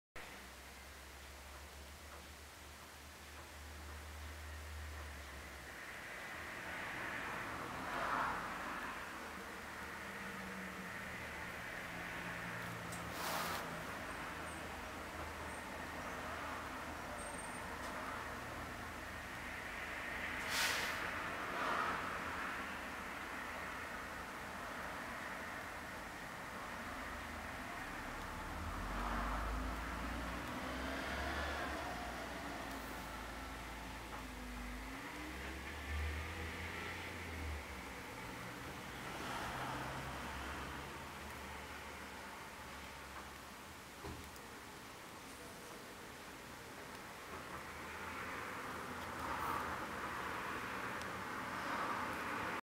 knife.wmv
Hand carving a leather belt using a swivel knife.